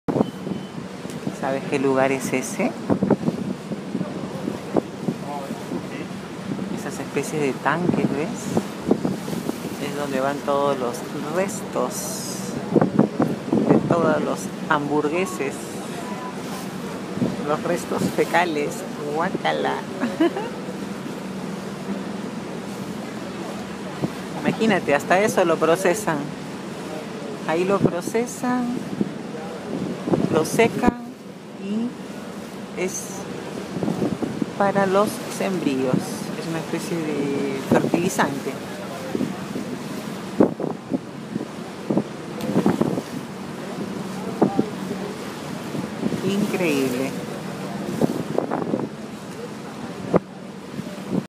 Tanques donde van los restos fecales de Hamburgo
Los procesan y sirven de abono, fertilizante!!!